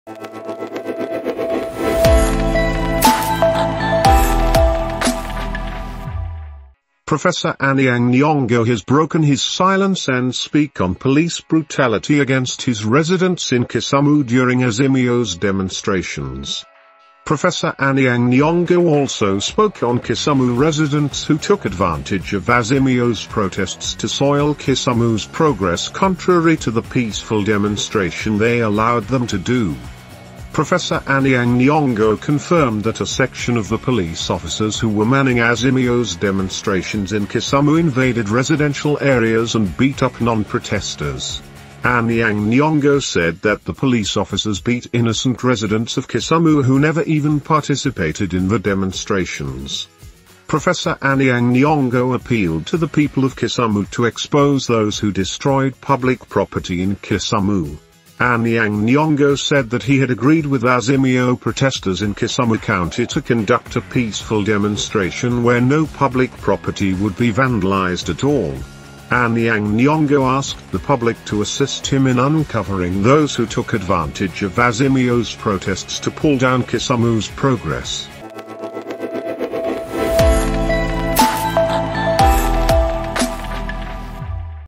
Anyang Nyong'o Deliver Bad News to All Kisumu Residents Involved In Anti Government Protest.
► The Official news54 Channel, Catch up with up-to-the-minute news and information from around the world.

©news54.2023